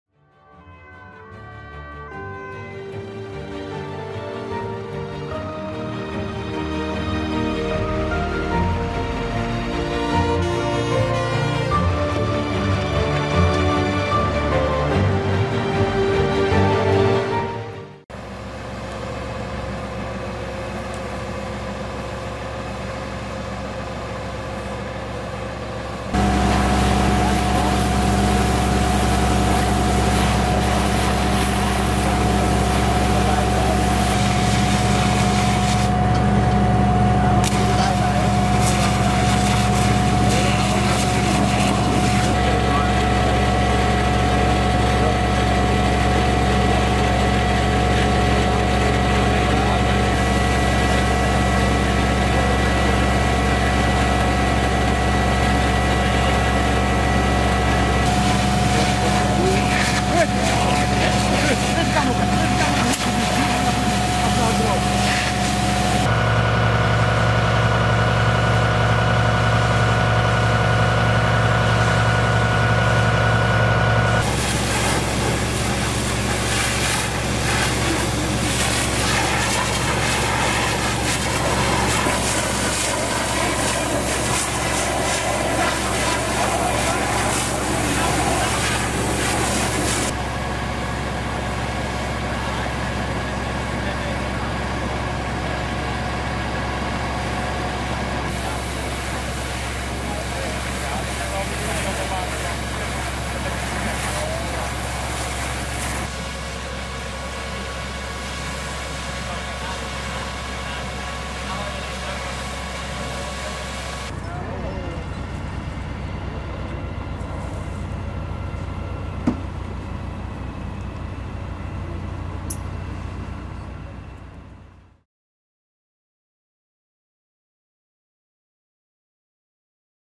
Brand in stapel hooi, Elspeterweg, Uddel.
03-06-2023 UDDEL - Omstreeks tien over half twaalf vanavond werd brandweer Uddel gealarmeerd voor een melding van een buitenbrand aan de Elspeterweg, voorbijgangers zagen een hooiberg in brand staan en belde de brandweer. De brandweer kwam ter plaatse met een tankautospuit en een waterwagen. Het koste de brandweer enige tijd om de brand te blussen omdat de berg deels uit elkaar moest worden getrokken om de brand te kunnen blussen, bij de brand kwam veel rook vrij. Nadat de brand was geblust keerde de brandweer terug naar de kazerne. De oorzaak van de brand lijkt te komen door broei.